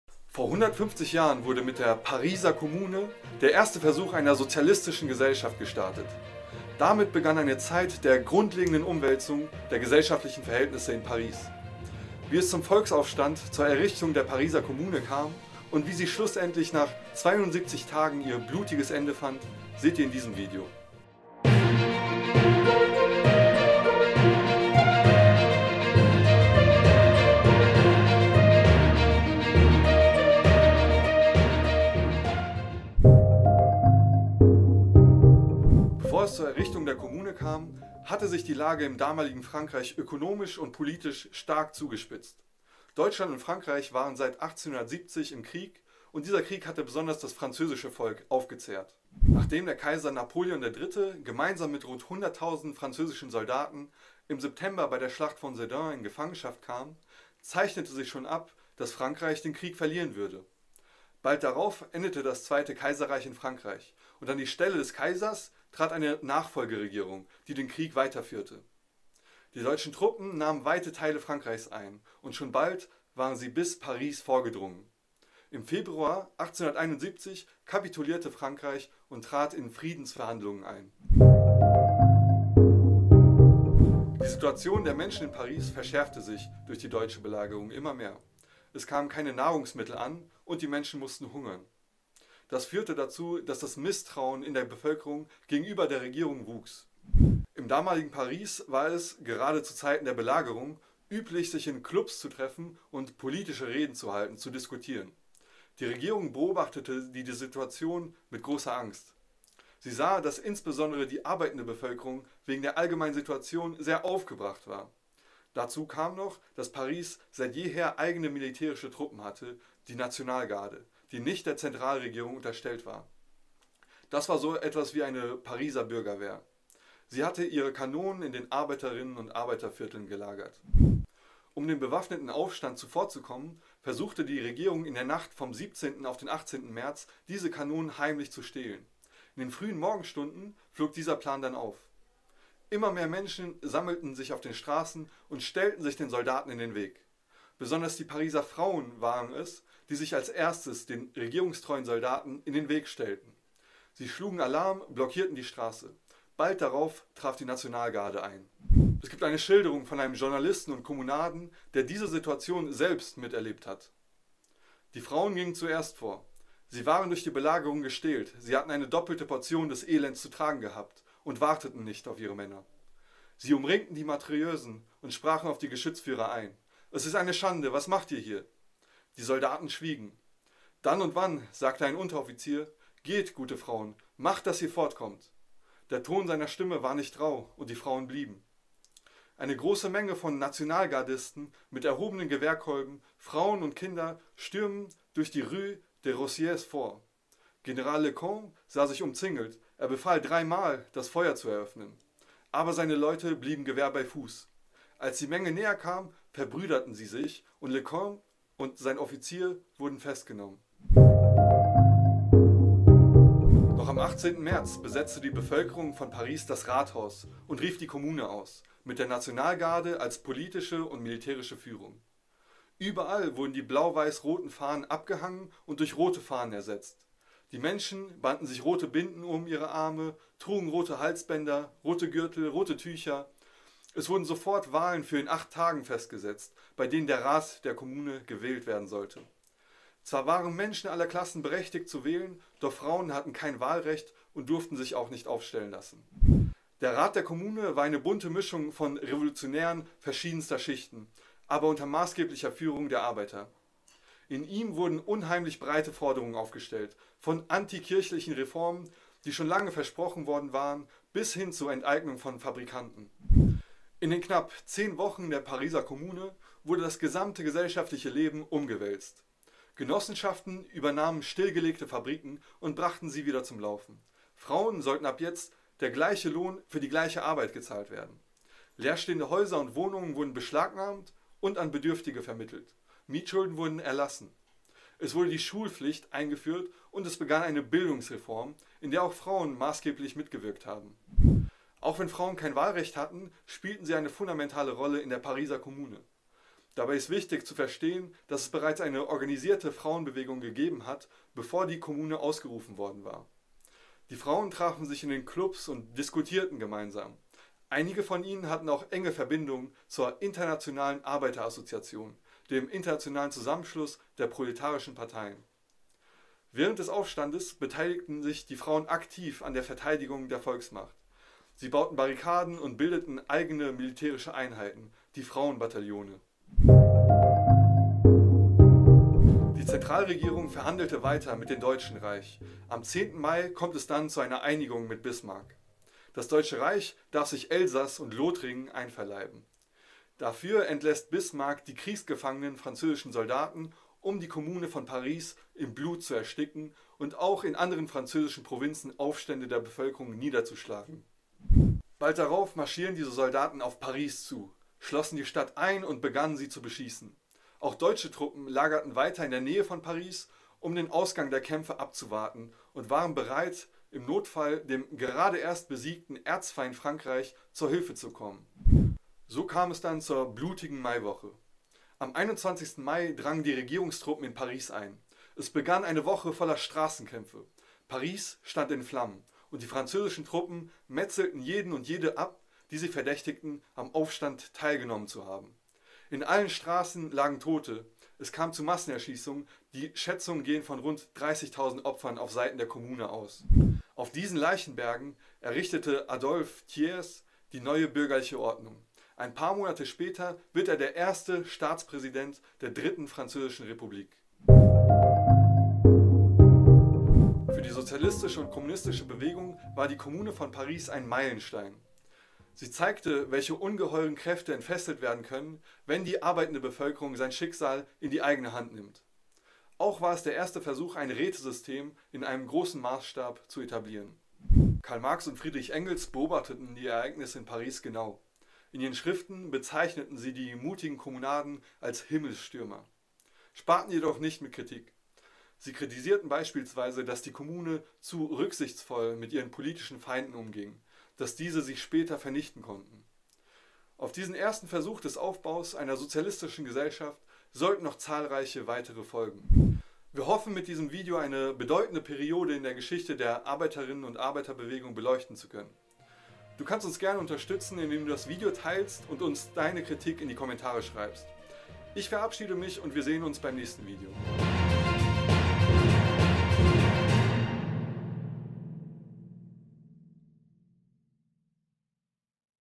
Was war die Pariser Kommune?
Vor 150 Jahren wurde mit der Pariser Kommune der erste Versuch einer sozialistischen Gesellschaft gestartet. Damit begann eine Zeit der grundlegenden Umwälzung der gesellschaftlichen Verhältnisse in Paris. Wie es zum Volksaufstand, zur Errichtung der Pariser Kommune kam und wie sie schlussendlich nach 72 Tagen ihr blutiges Ende fand, seht ihr in diesem Video.

0:00 Intro
0:33 Vorgeschichte
1:21 Beginn des Aufstands
3:31 Das revolutionäre Paris
5:27 Die blutige Maiwoche
6:45 Die Internationale Arbeiter:innenbewegung und die Kommune